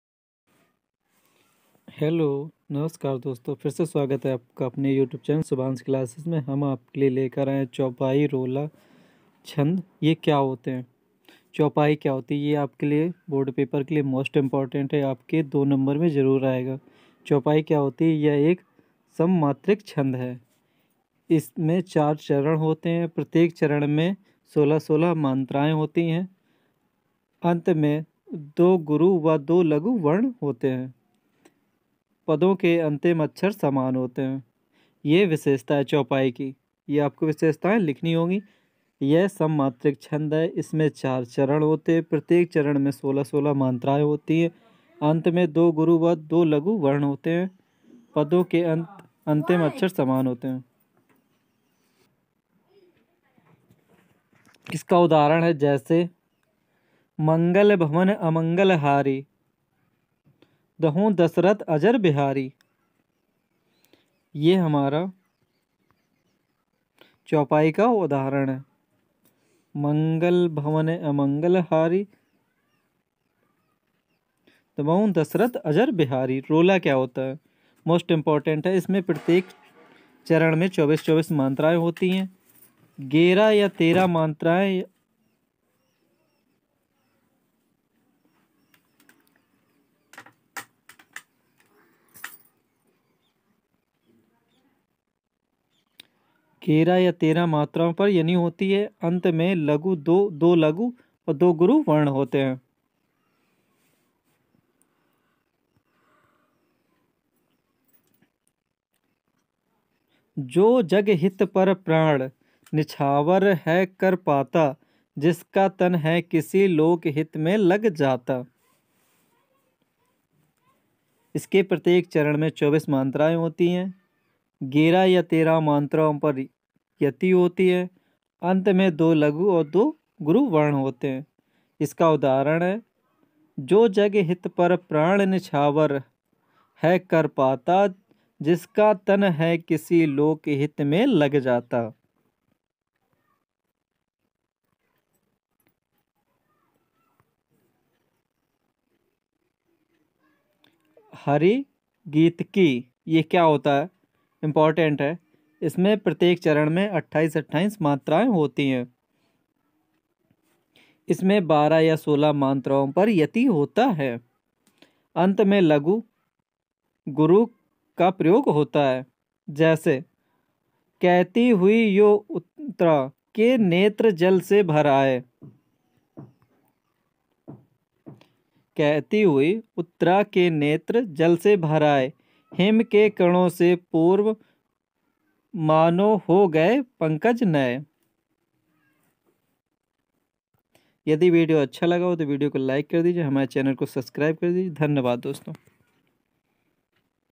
चौपाई एवं रोला तथा हरिगीतिकी क्या होते हैं इन्हें उदाहरण सहित समझाइए
रोला चौपाई क्या होते हैं इनको हमने उदाहरण सहित बताएं जो आपके लिए पेपर की दृष्टि से मोस्ट इंपोर्टेंट है तो अब इस पर जरूर पूरा देखेगा धन्यवाद दोस्तों